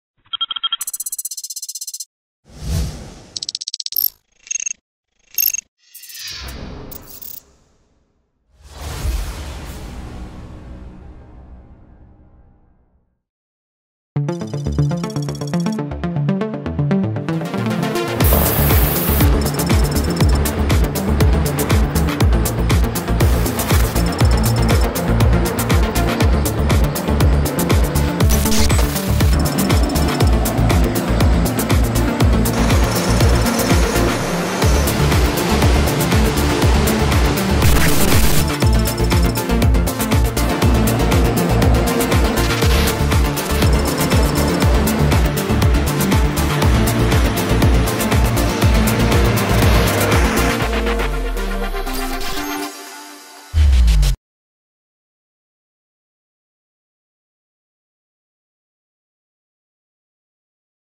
Ship-to-Shore Connector
Navigating the world’s shorelines just got easier with the Ship-to-Shore Connector. The SSC is the next generation of the Landing Craft, Air Cushion (LCAC), providing a modernized means to reach the world's shorelines for the next 30 years. The all-aluminum craft offers increased performance, reliability and maintainability and meets the Marine Corps’ requirements for increased payload and availability.